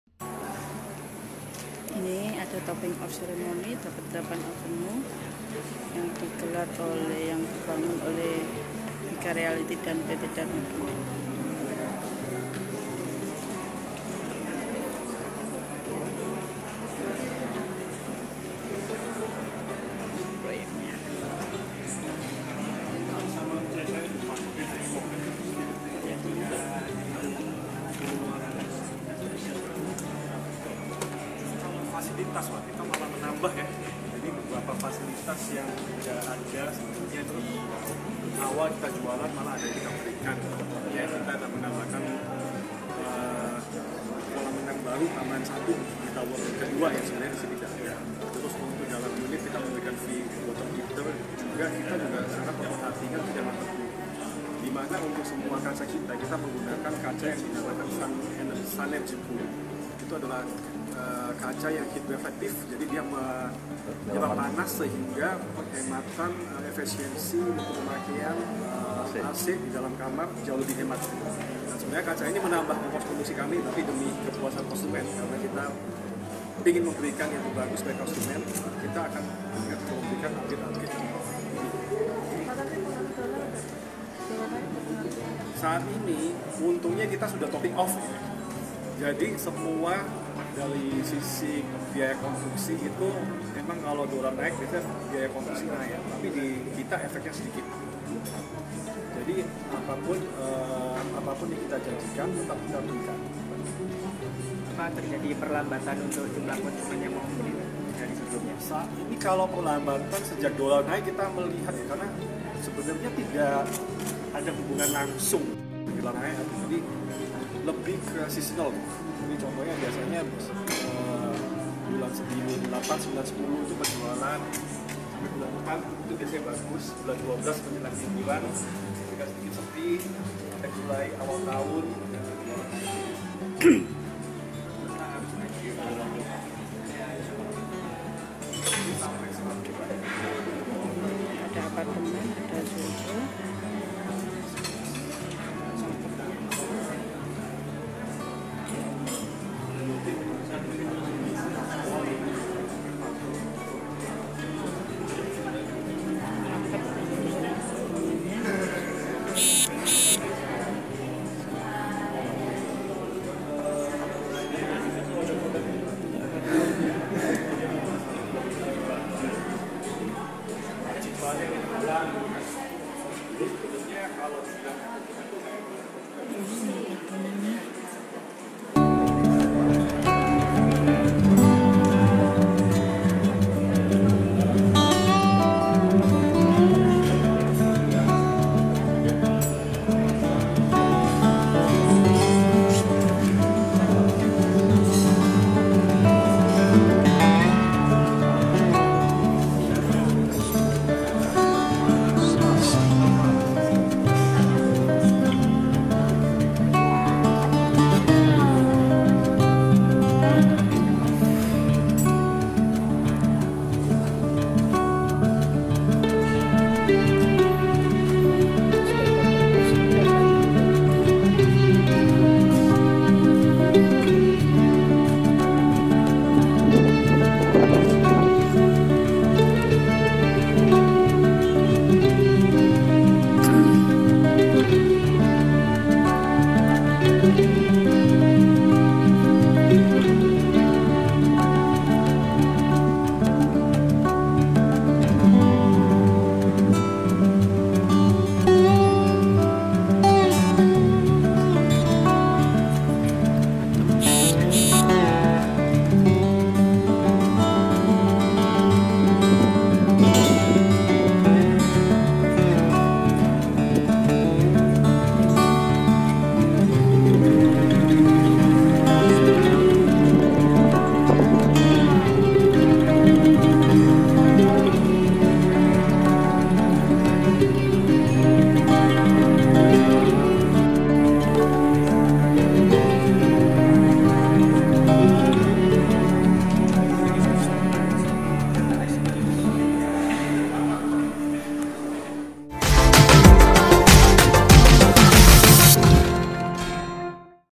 Superblok 88 Avenue Telah Mencapai Topping Off
SURYA Online | Proyek kolaborasi antara BUMN Kontruksi PT Waskita Reality dengan PT Darmo Permai, superblok 88 Avenue telah mencapai topping off untuk dua dari delapan tower yang direncanakan. Topping off digelar Rabu (10/10/2018) dengan target serah terima Agustus 2019 mendatang.